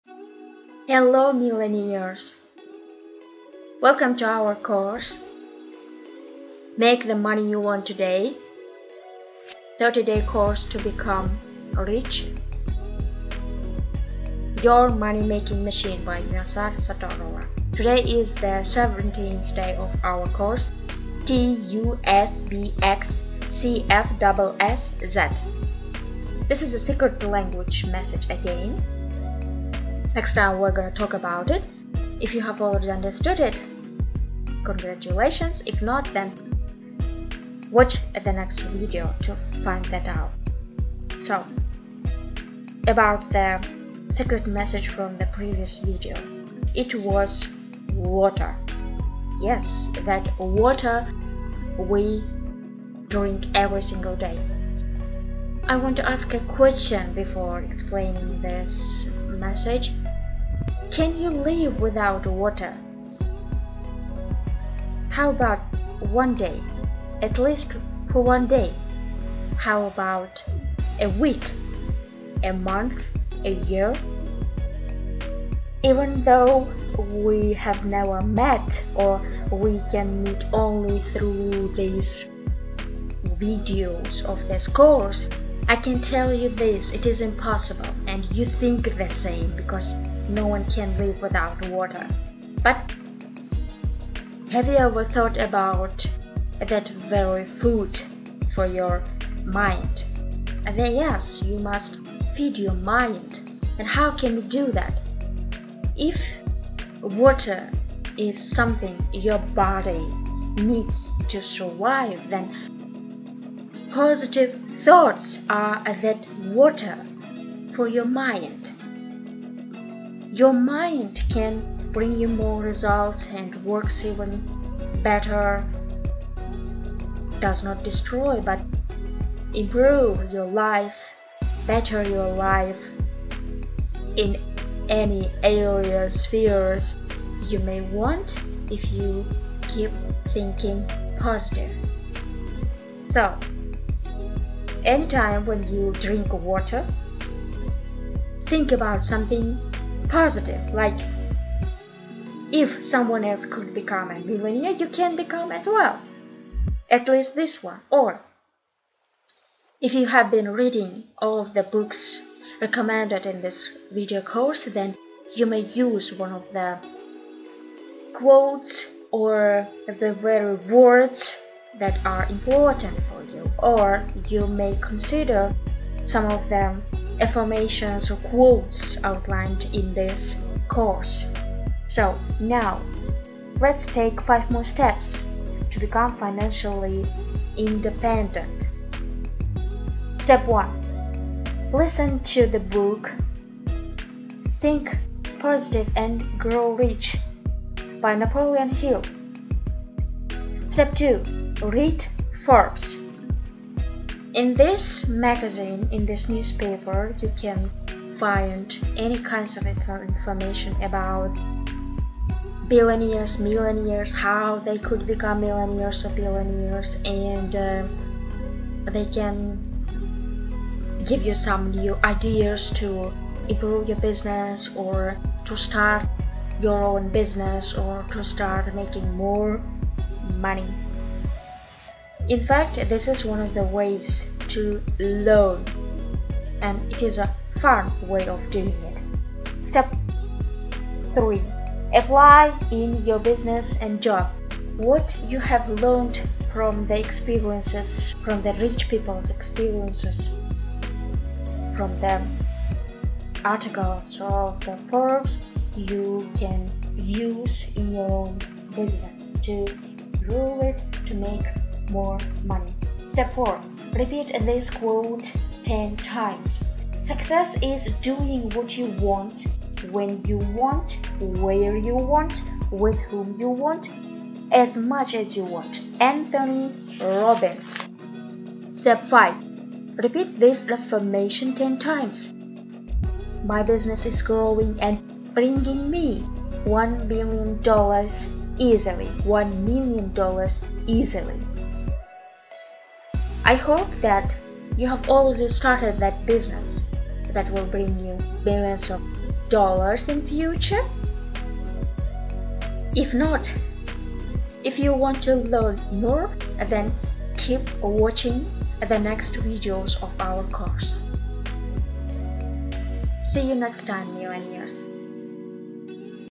Make the Money You Want Today Your Money Making Machine 17 | This video Will Make You RICH
|Manifest money easily  |Millionaire Mindset |Millionaires` habits that can make you rich After watching this, your mindset about money will change!

Hello, Millionaires!

How do the Rich think and work to make their fortunes? They know something we do not! In this video, we will discover that secret weapon that all wealthy people use to make money. Watch all the videos and you will start making money without working. Because, money will  work for you! You will become a magnet for wealth. All you have to do is to watch all of these 30 videos and follow the instructions and act upon your dream!